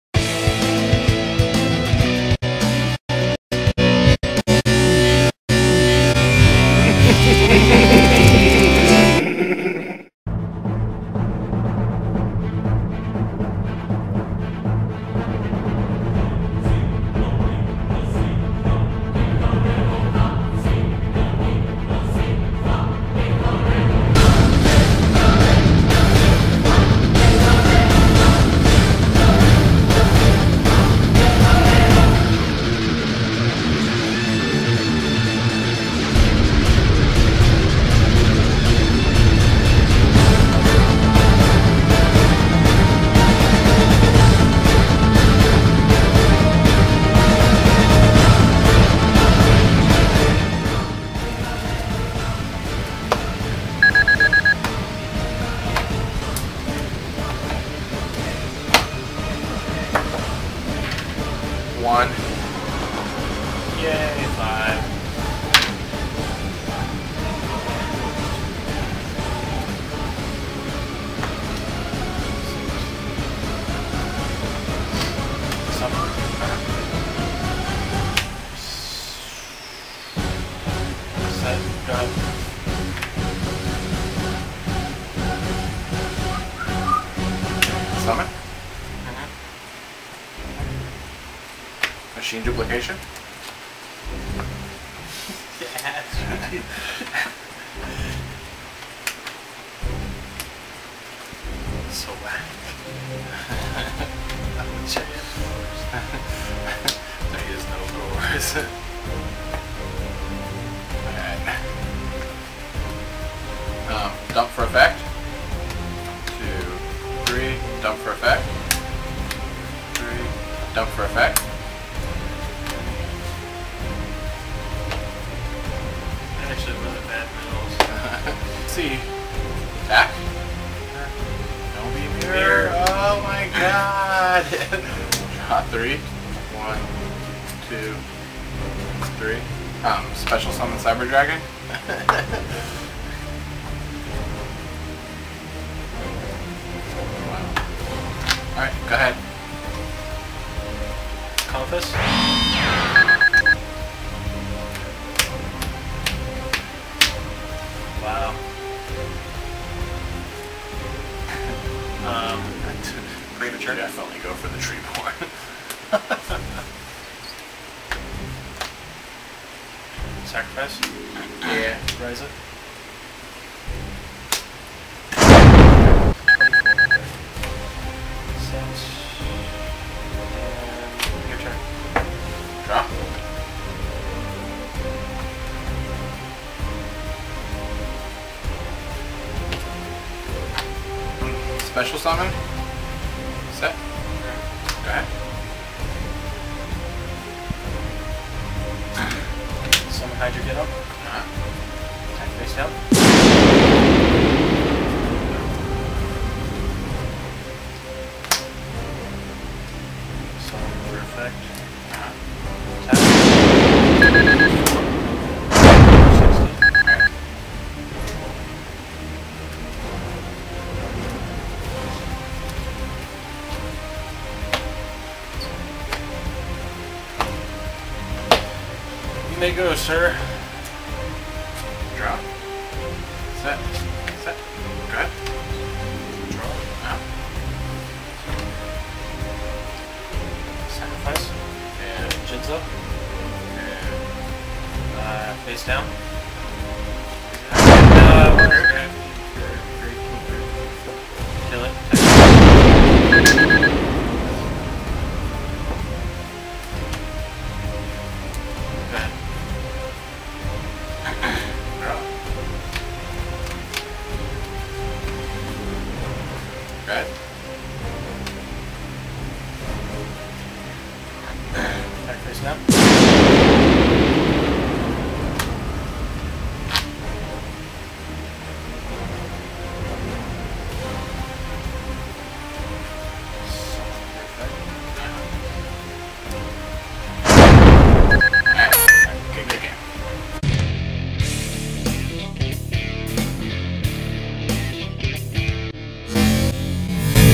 Yugioh Old School Duel - Format: March 2007 - Round 1
Follow the journey of two brave souls as they return to battle it out in formats of the past. The rules: They must only use cards released up till that point and they must use the Limited/Restricted list that was in use at that time.

Format Used:
March 2007
Round #: One